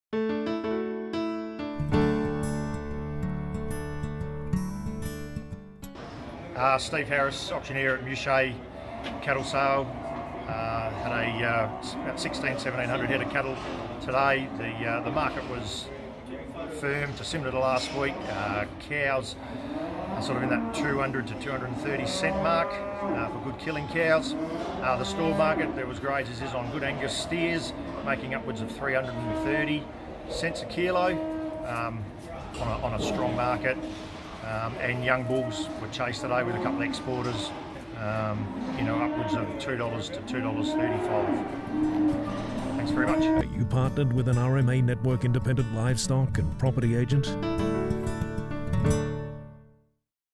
Muchea Weekly Cattle Sale, Monday 30th September 2019
We caught up with Steve Harris of Westcoast Wool & Livestock for the market report from the Muchea Weekly Cattle Sale, held Monday 30th September.